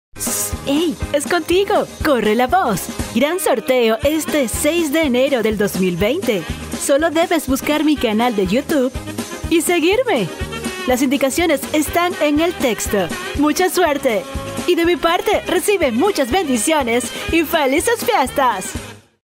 GÁNATE UNA GIFT CARD - 6 ENERO 2020
🌟 CONCURSO 🌟 ¡GÁNATE una digital card el 6 de ENERO del 2020!
💡participa en el debate (o comentarios) de mí canal e invita a un amigo a participar...
😅 Pero sí quedas seleccionado deberas seguirme en Instagram -Twitter y Facebook Búscame así  @moraopaula y haber reposteado el video y post de la promo de este sorteo.
🤩 El sorteo se hará con la lista de seguidores de mi canal de YouTube y el sorteo lo haré EN VIVO así que pendientes (Igual les estaré avisando)
En YouTube si me sigues, es importante que actives tus suscripciones de forma pública; los que las tienen privadas no aparecerán en la lista a pesar de haberse suscrito a a mí canal. ( por lo menos actívalo público ese día del sorteo)